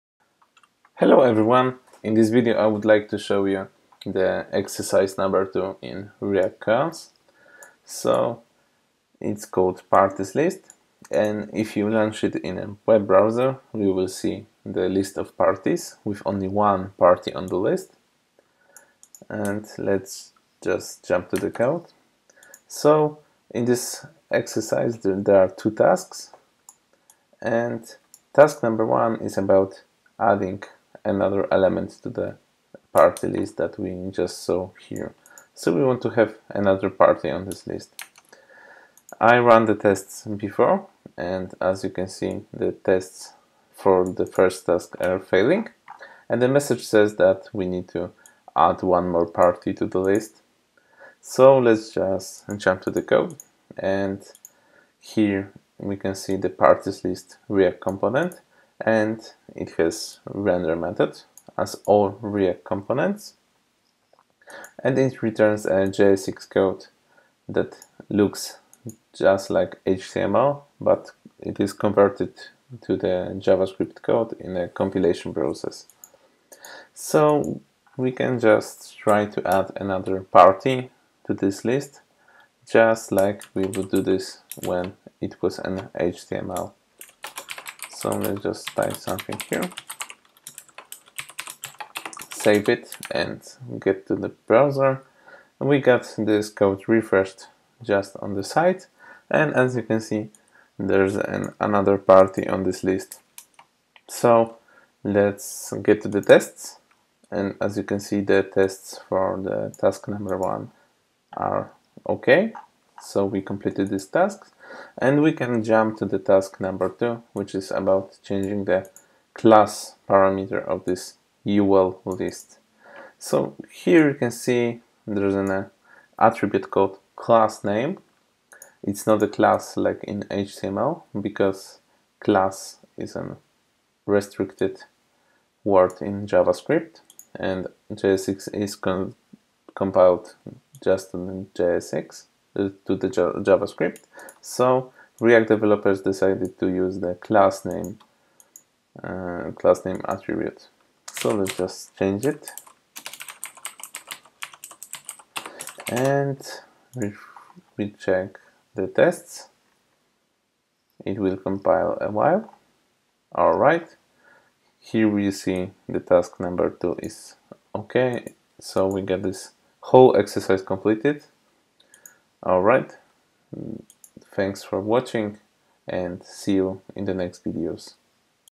React.js koans - solving the 2nd exercise, the list of parties!
Learn React.js by practice.